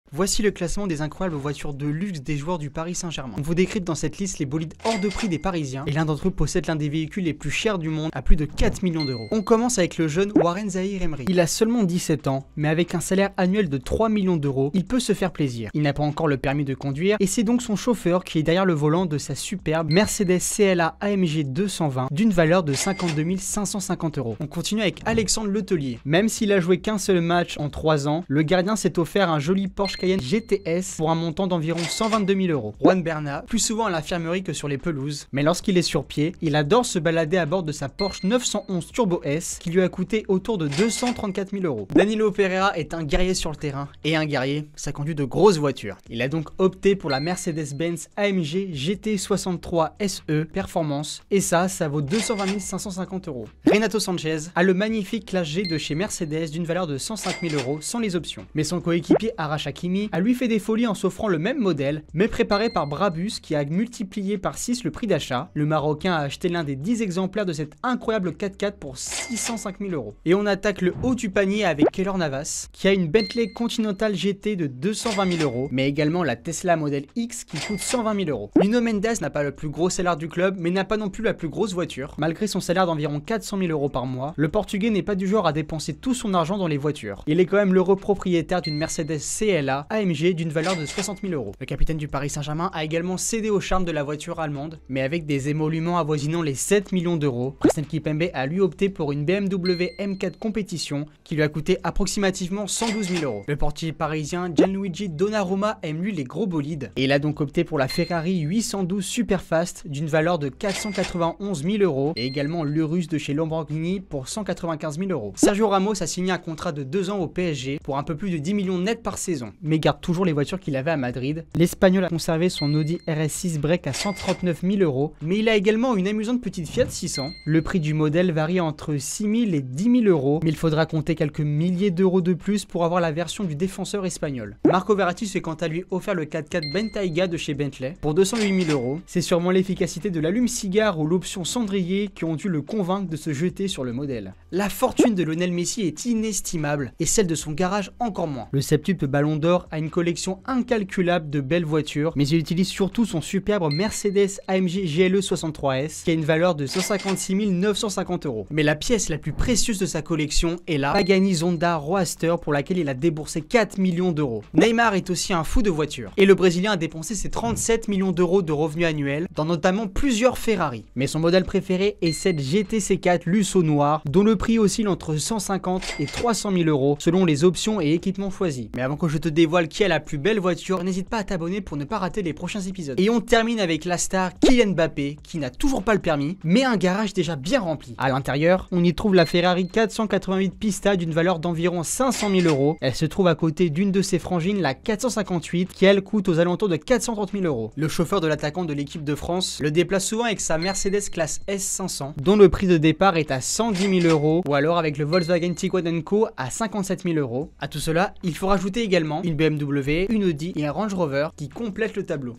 Le prix du bolide qu'a offert Mbappé à son chauffeur 😱
Tous les modèles et prix des voitures de joueurs du Paris Saint-Germain 😱